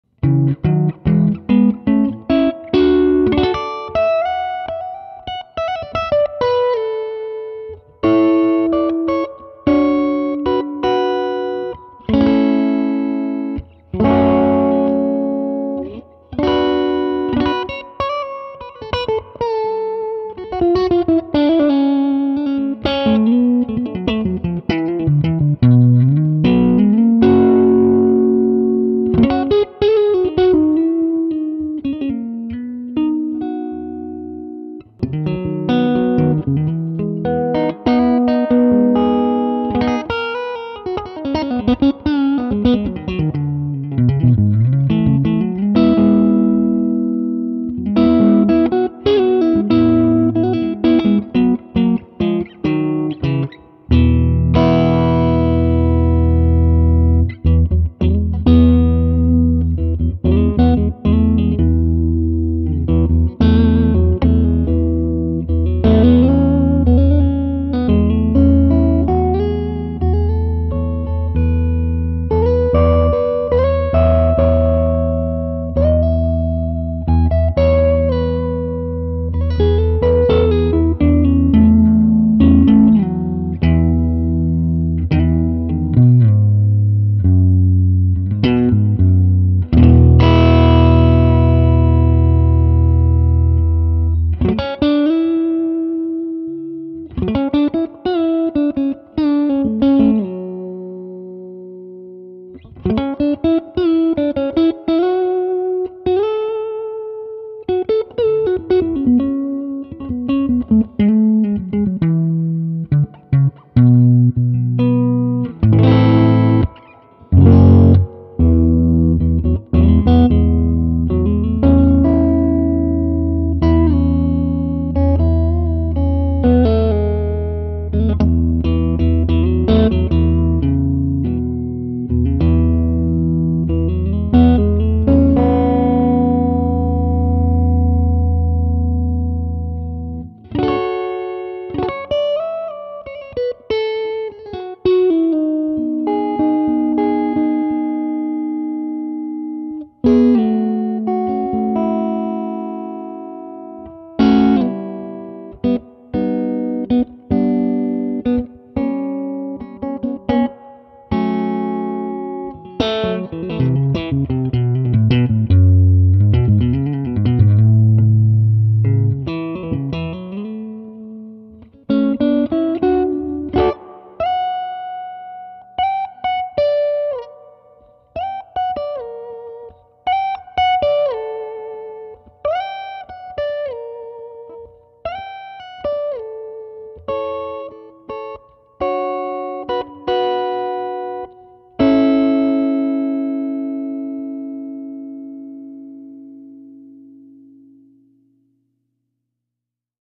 3 Minutes with a Benson Nathan Junior and a Stratocaster
3 Minutes with a Benson Nathan Junior and a Stratocaster
Absolutely beautiful sounding amp. Mic'd with a Cascade Fathead. Squier strat.